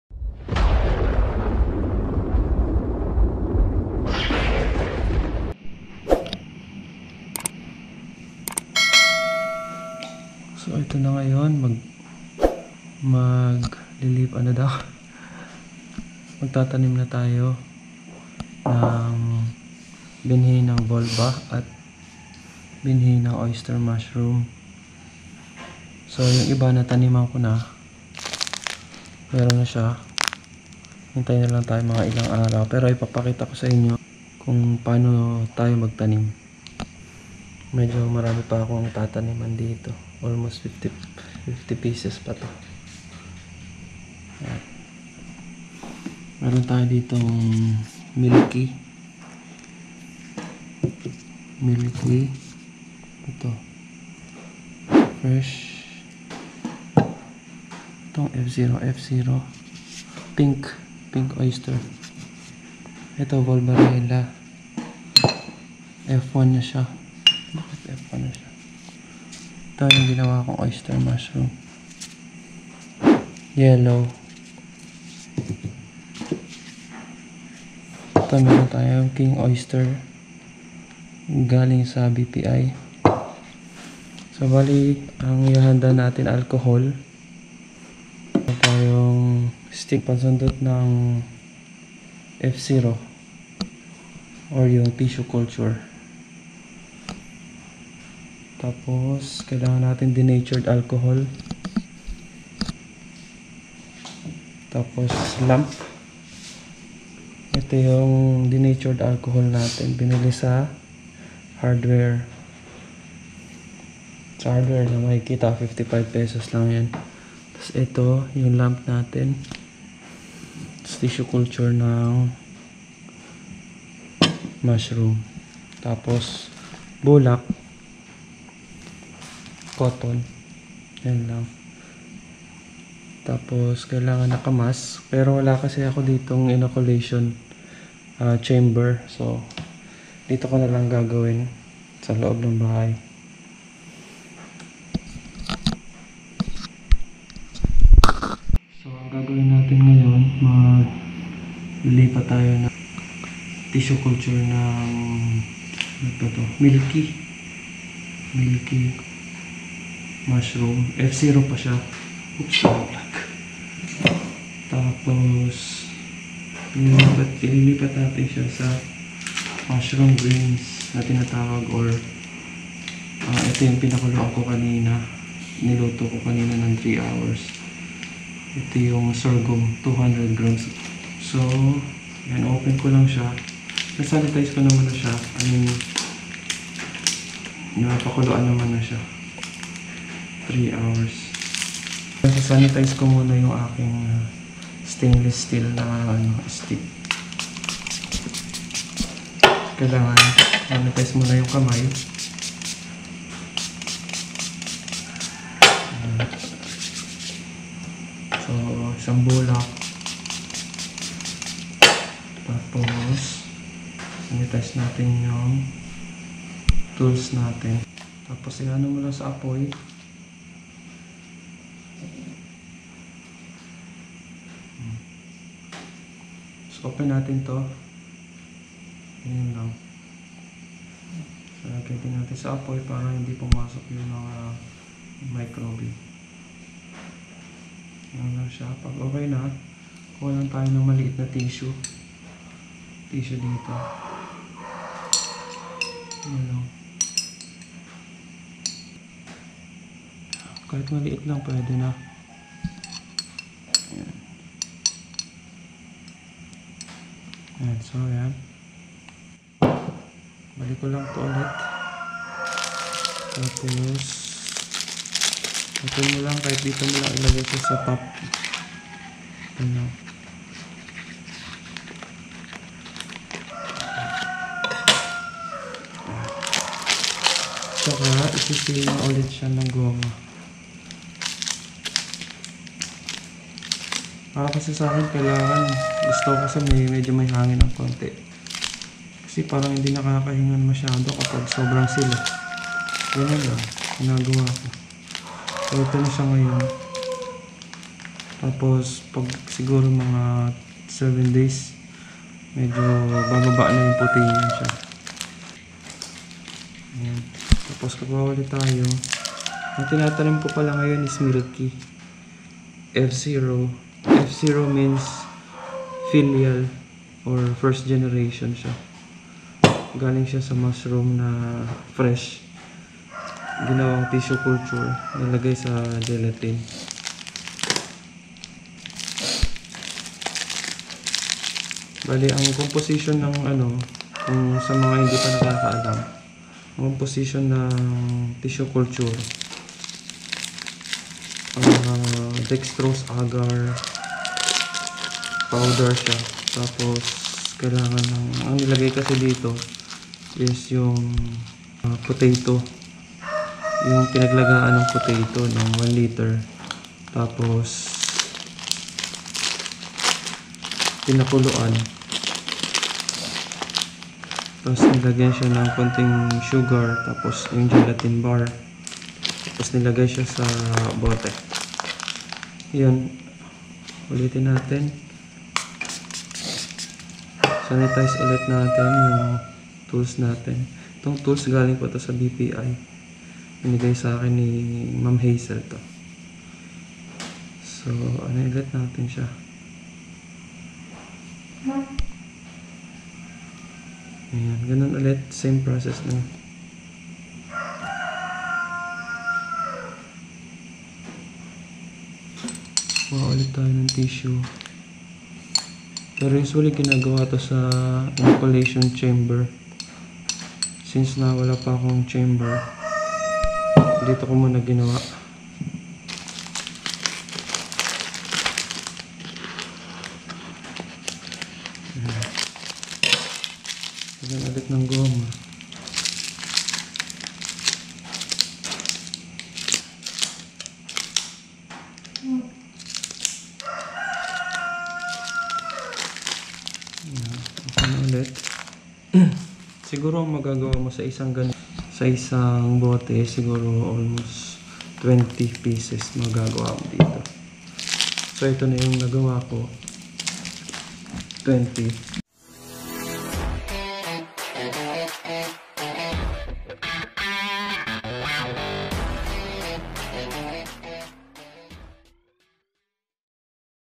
pano gumawa ng binhi ng mushroom .. tissue to grains
ituturo ko dito kung paano itanim ang tissue culture na mushroom sa isang sorghum grains